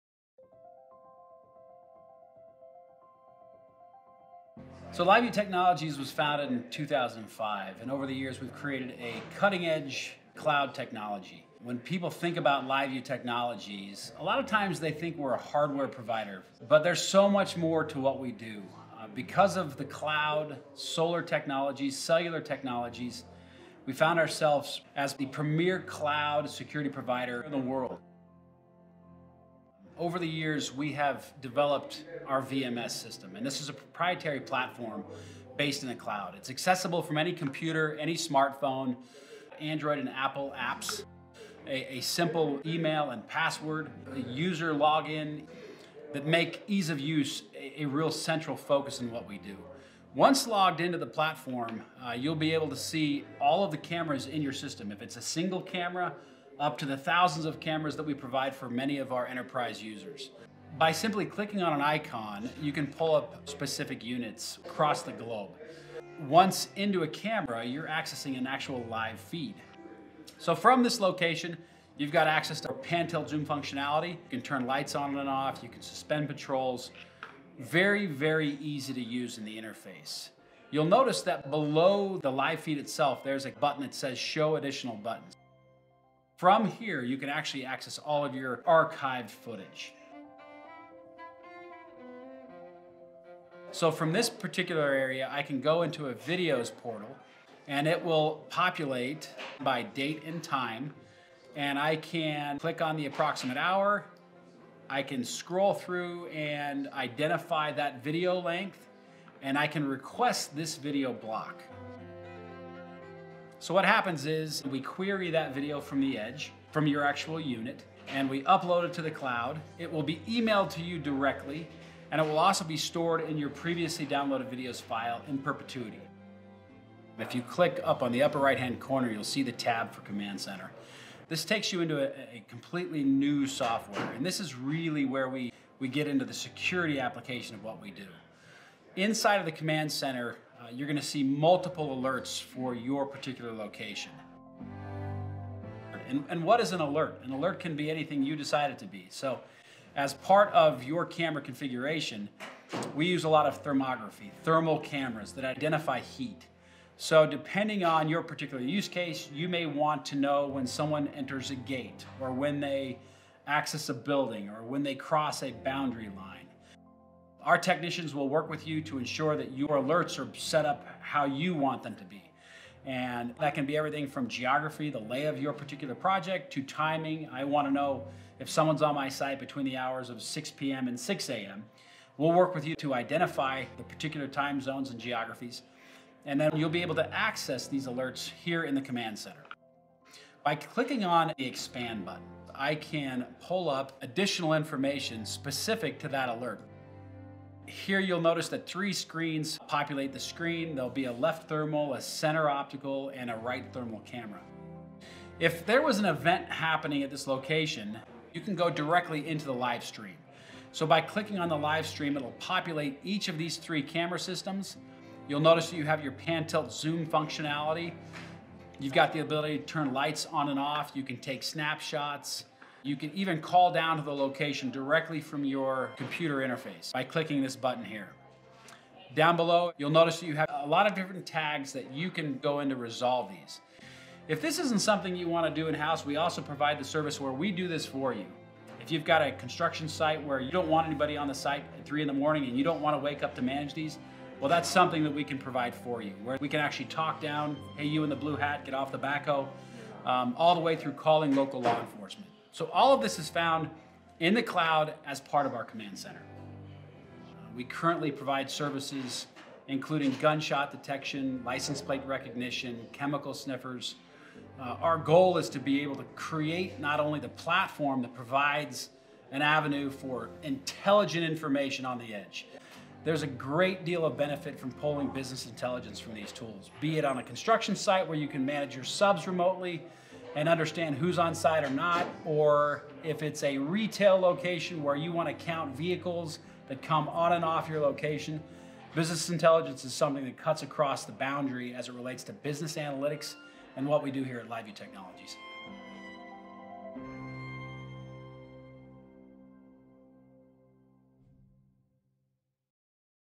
Learn about the LiveView Technologies VMS & Command Center from David Studdert
The LVT platform is easy-to-use, completely remote, and packed full of functionality. It allows our customers to detect, deter, defend with fully autonomous safety and security with LiveView Technologies mobile security trailers.